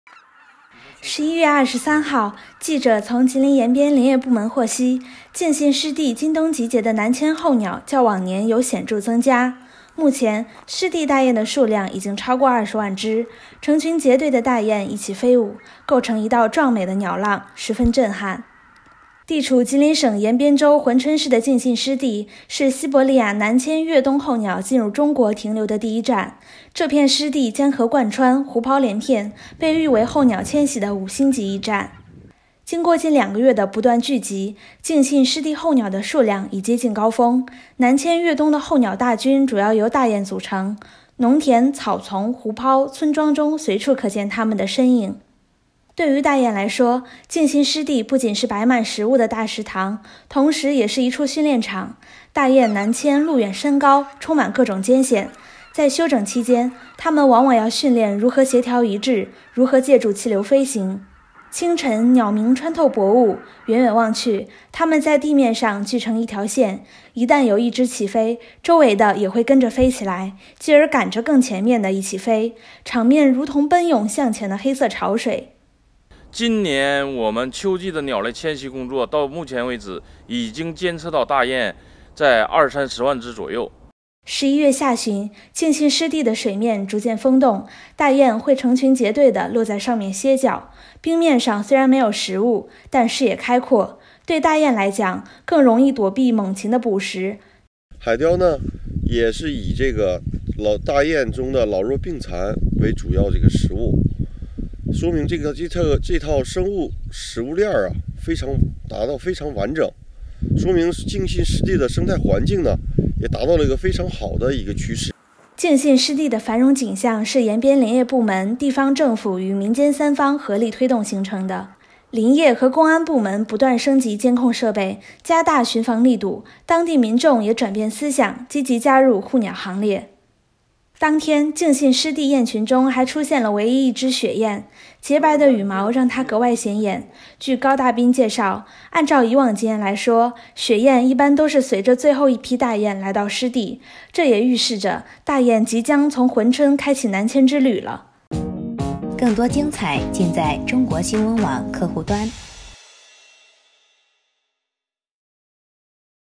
吉林珲春敬信湿地现万重“鸟浪” 场面壮观
地处吉林省延边州珲春市的敬信湿地，是西伯利亚南迁越冬候鸟进入中国停留的第一站。这片湿地江河贯穿、湖泡连片，被誉为候鸟迁徙的“五星级驿站”。
        经过近两个月的不断聚集，敬信湿地候鸟的数量已接近高峰。南迁越冬的候鸟大军主要由大雁组成，农田、草丛、湖泡、村庄中随处可见它们的身影。